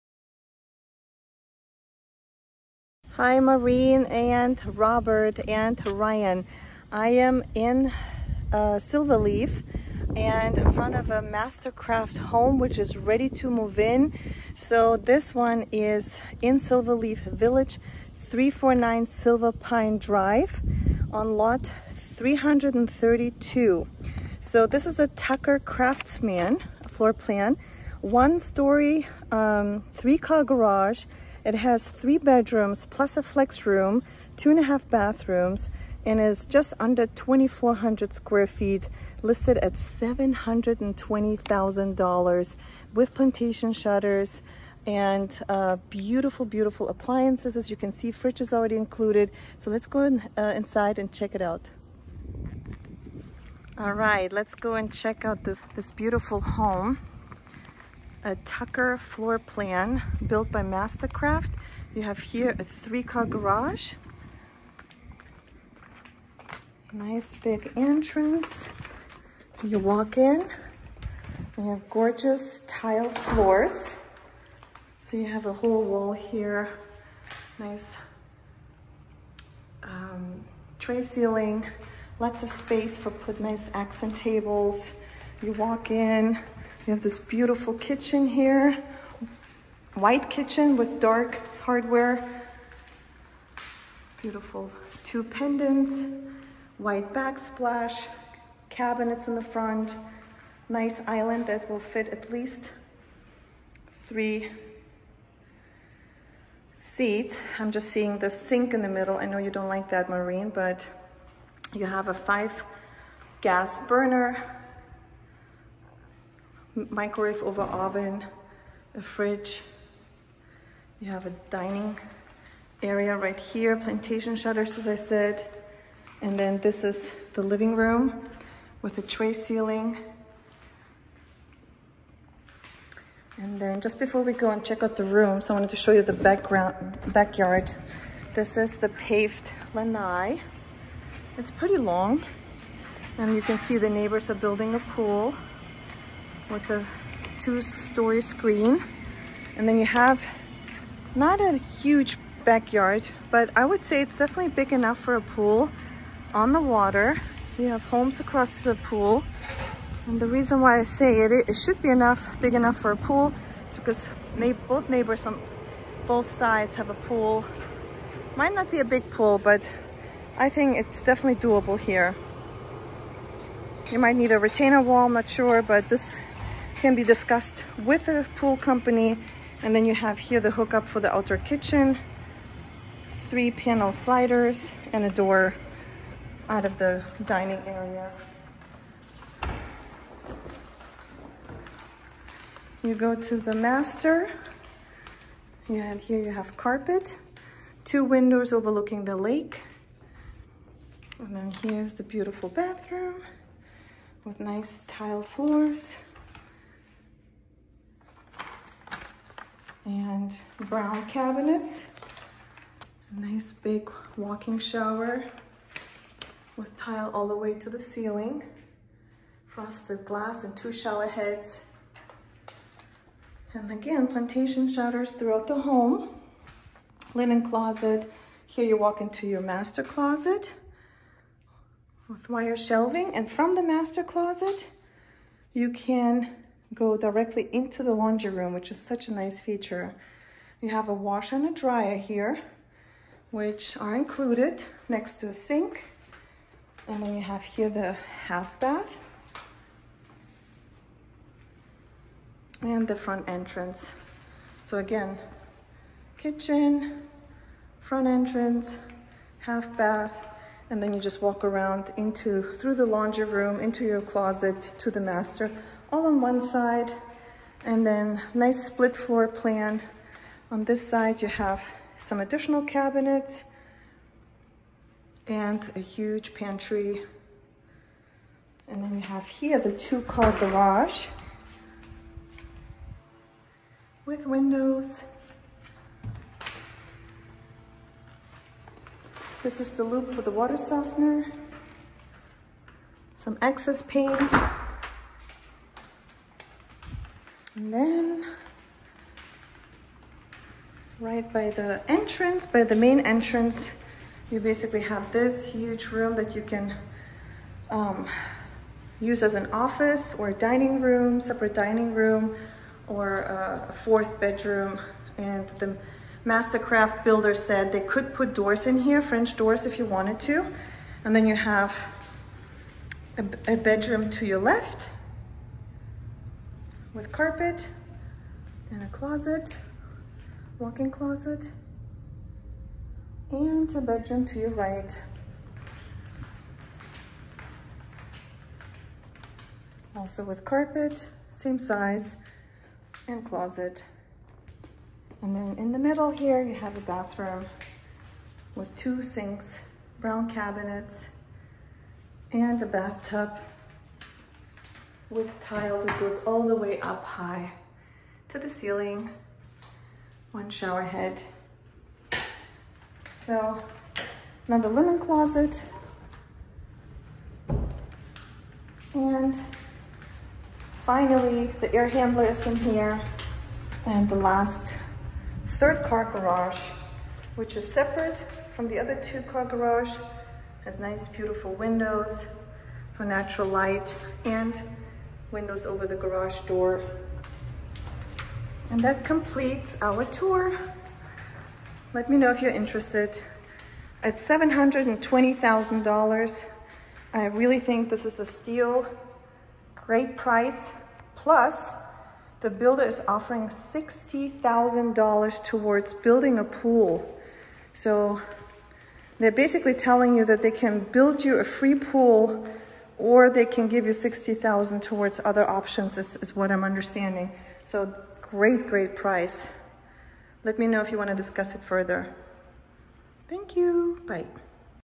SILVERLEAF MOVE-IN-READY HOME FOR SALE - 8/22/2023
🏡 Ready to Move? Discover Your New Home in Silverleaf TODAY!
🏠🌳 Explore this stunning Tucker floorplan by Mastercraft Building Group – a move-in-ready masterpiece with 3 beds, 2.5 baths, and 2,354 SF of elegance. Priced at an incredible $720,000, it's your chance to embrace luxury living. But that's not all! MasterGroup sweetens the deal with an unbeatable offer: a FREE pool build or $60,000 credit towards one. Dive into your dream home and a refreshing pool paradise! 🌟🏊‍♂️